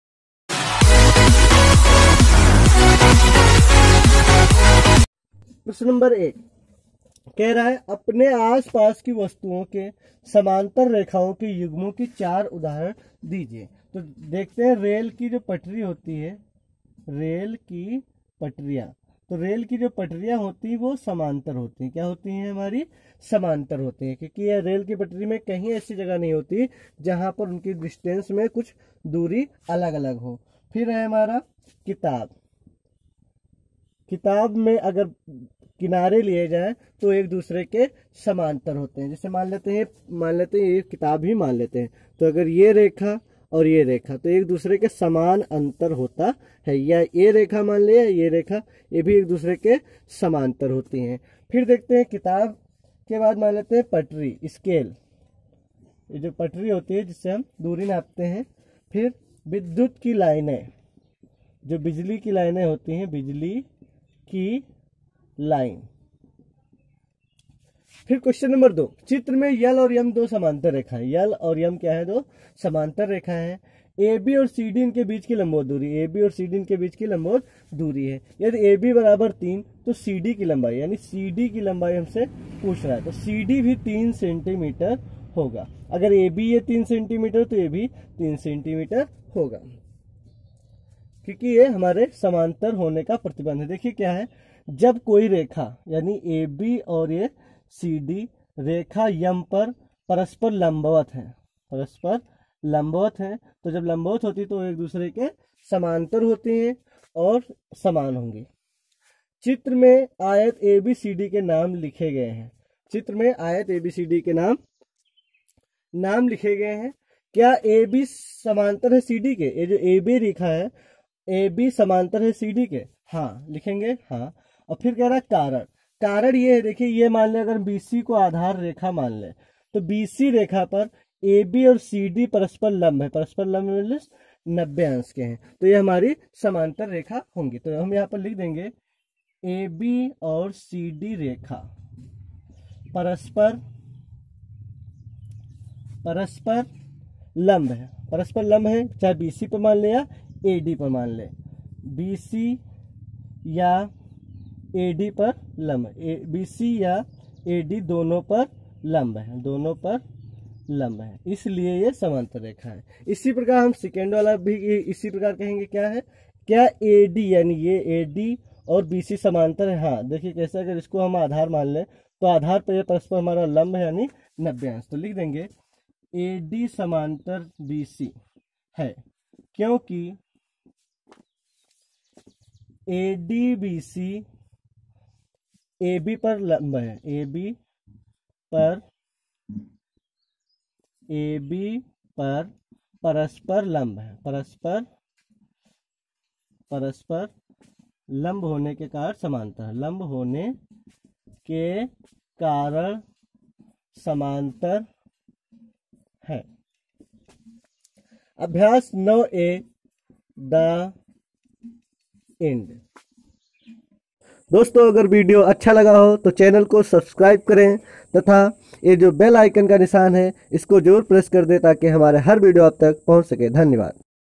CLASS 6th MATH exercise 9(a)
कक्षा 6th गणित हिन्दी माध्यम
Class 6th MATH Hindi medium
अभ्यास 9
Exercise  9
समांतर लंब और समांतर
दो सरल रेखाओं और एक त्रियक रेखा द्वारा बने कोण
पटरी गुनिया